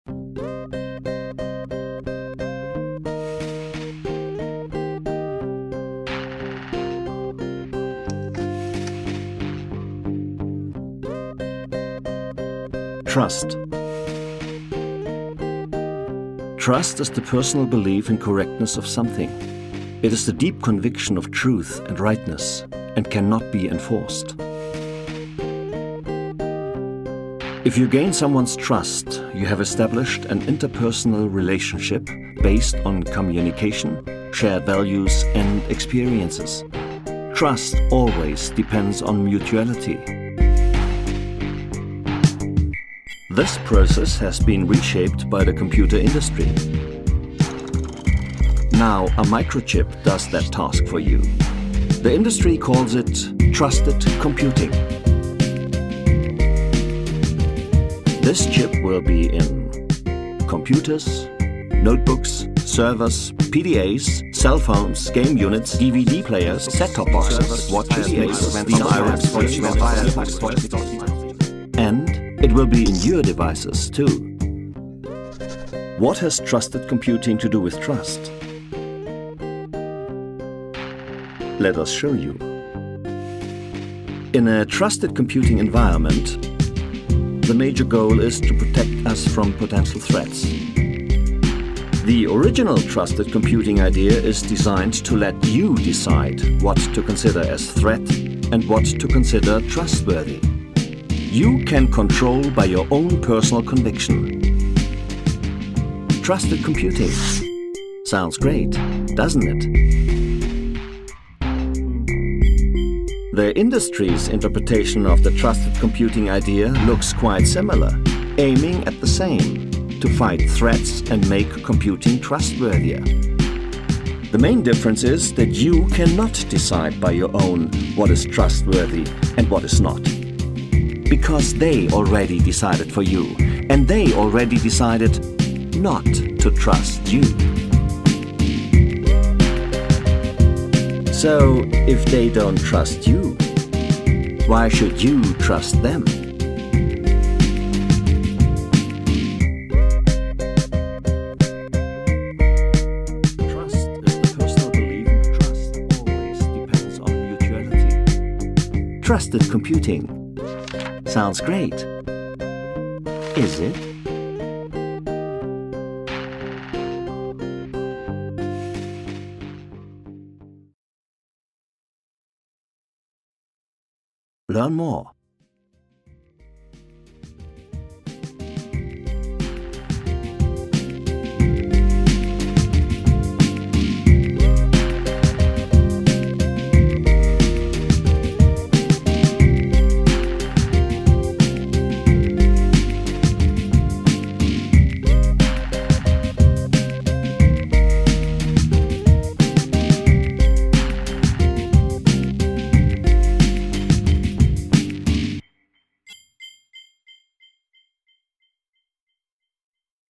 Trusted Computing (Legendas em Português Brasileiro)
Ótimo vídeo sem jargão técnico que explica o que significa "Trusted Computing".

Vídeo em Inglês, com legendas em Português do Brasil.

Disponibilizado sob uma variante de licença Creative Commons.